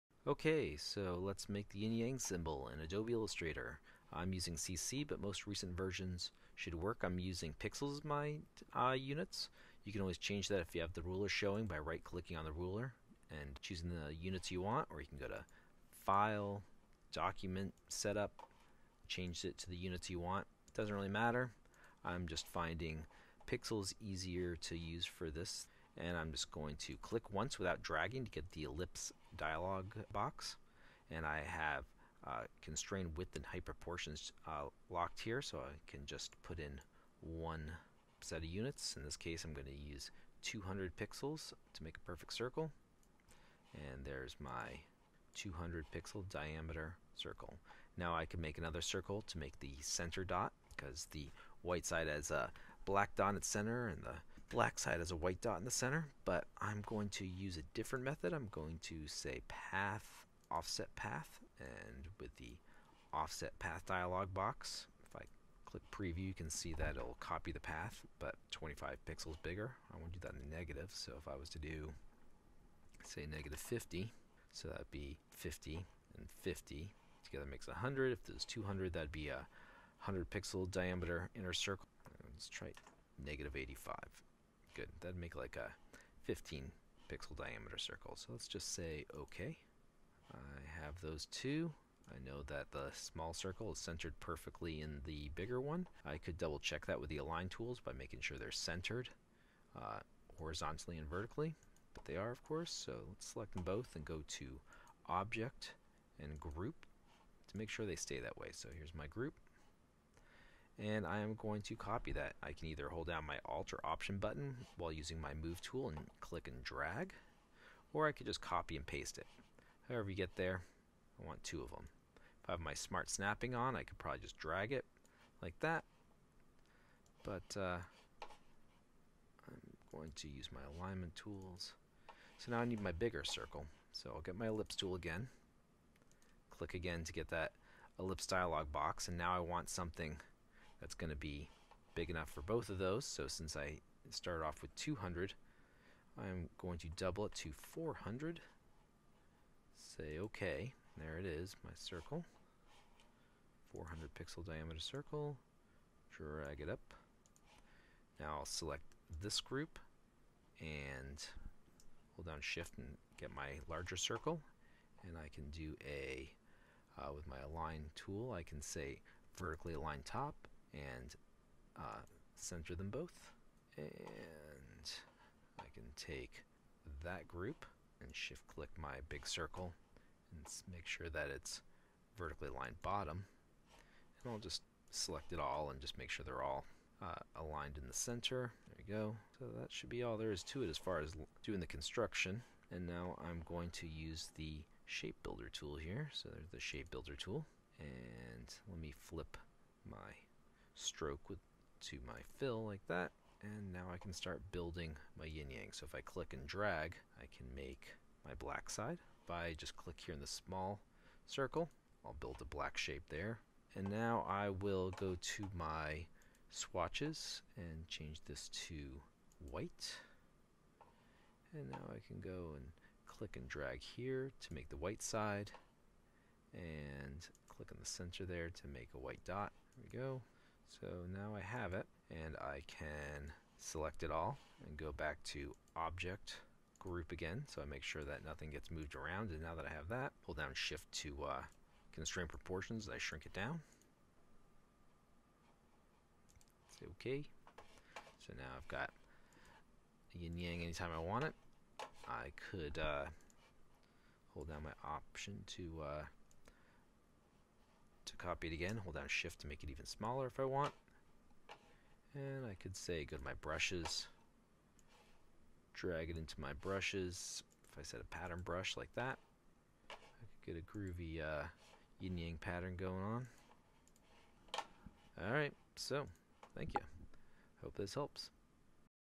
Adobe Illustrator tutorial, make a Yin Yang symbol
A quick (for me) tutorial making a yin yang symbol using the shape builder tool and some very basic math